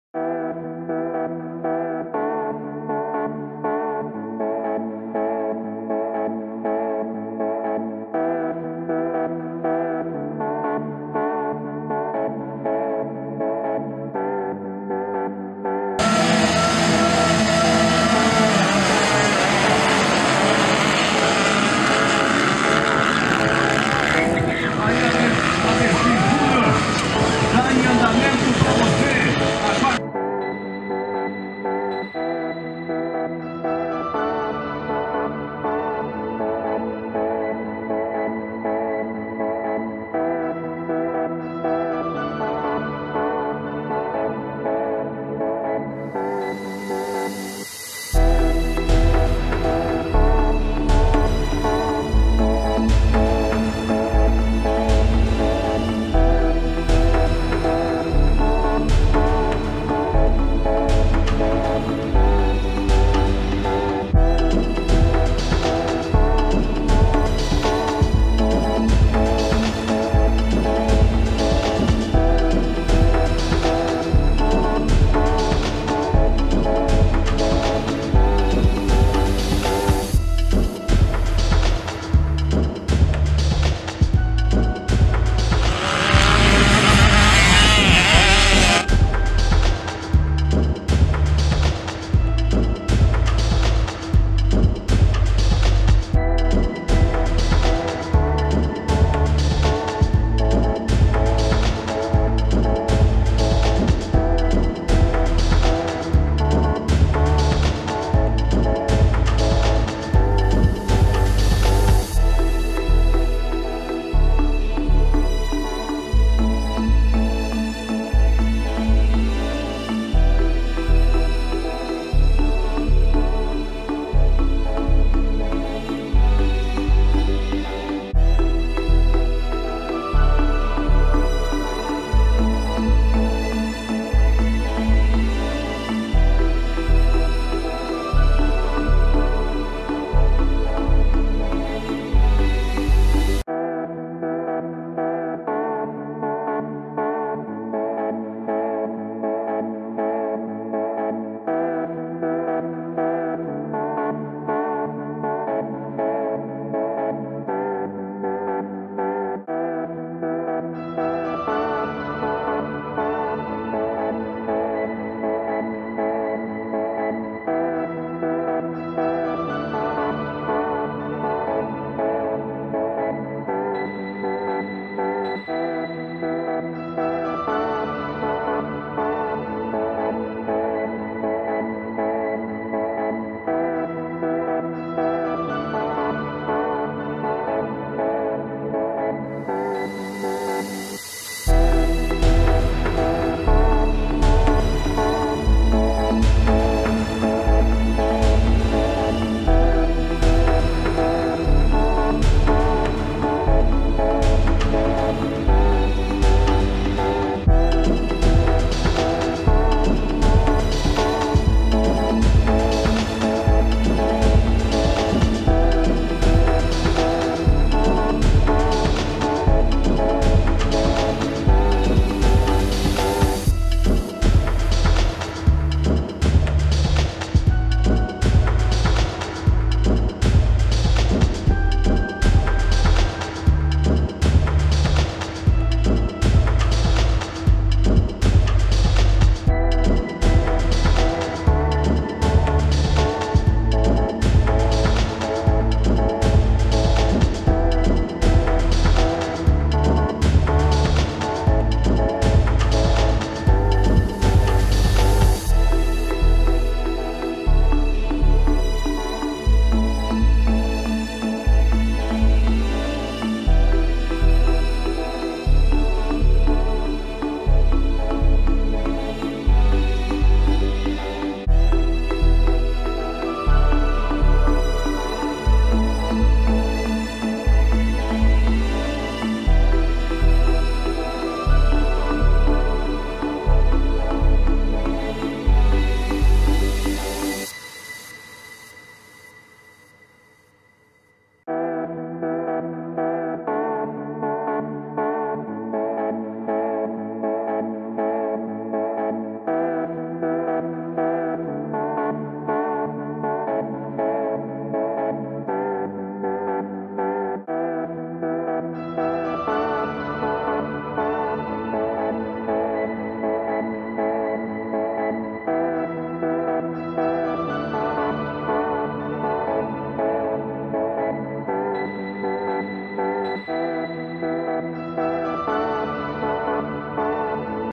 Egídio Gabriel leva melhor na VX Júnior em Caarapó
4º Etapa do Campeonato Estadual Sul-mato-grossense de Velocross 2022

Caarapó (MS)

Imagens @42heliolima